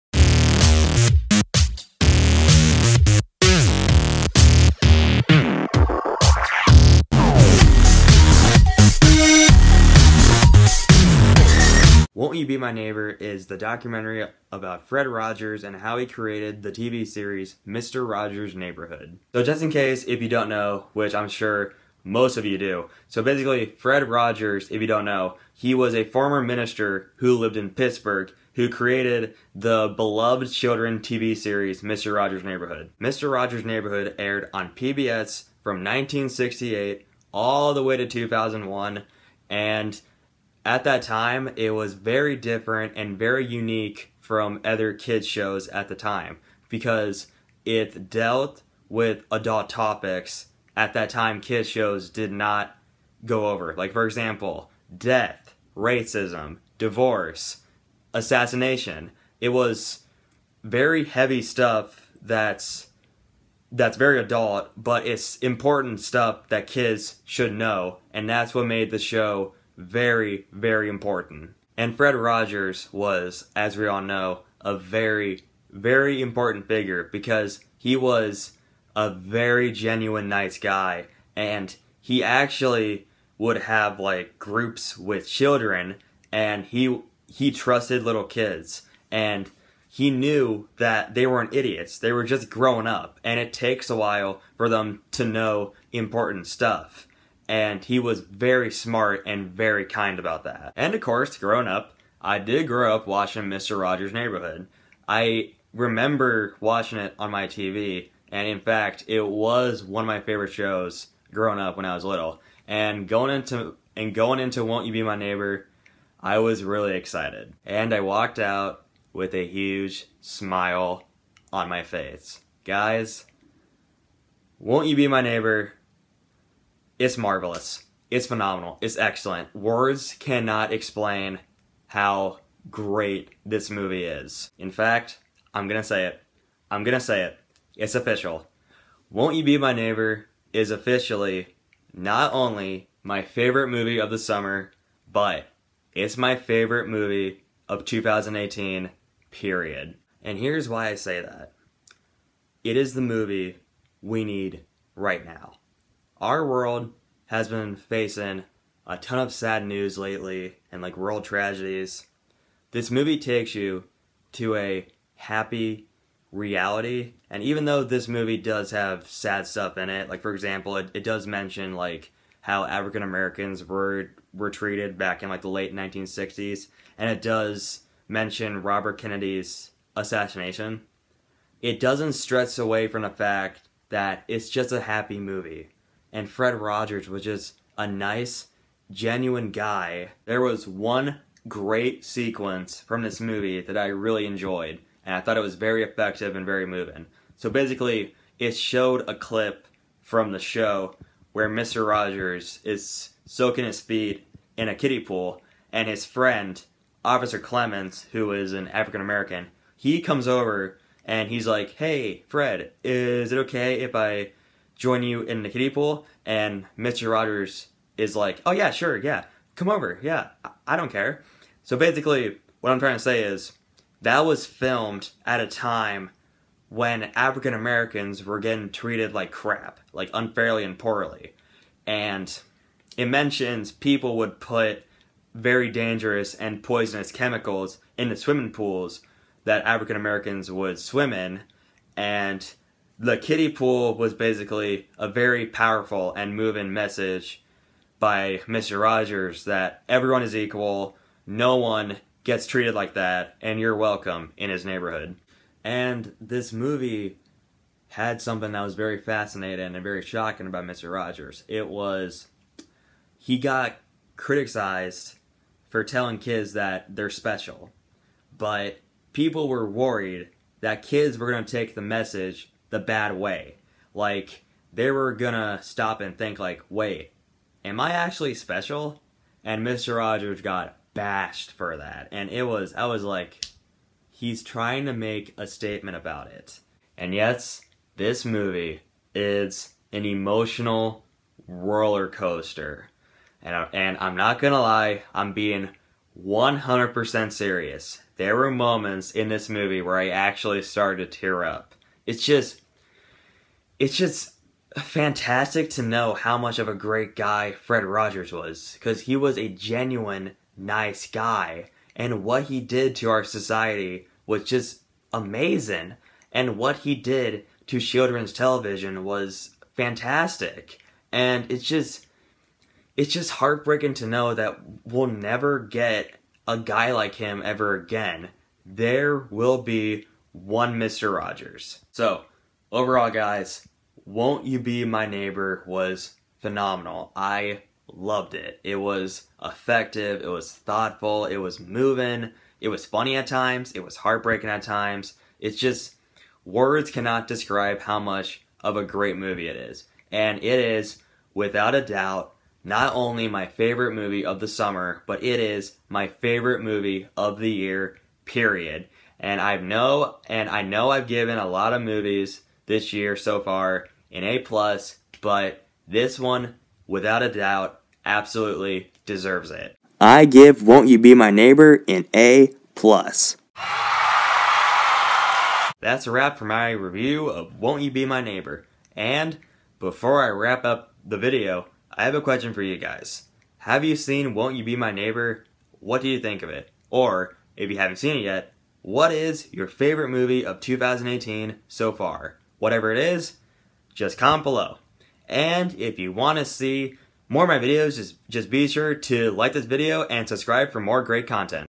Won’t You Be My Neighbor? - Movie Review
A documentary about Fred Rogers and how he created Mister Rogers’ Neighborhood, one of the most beloved and important TV shows of all time.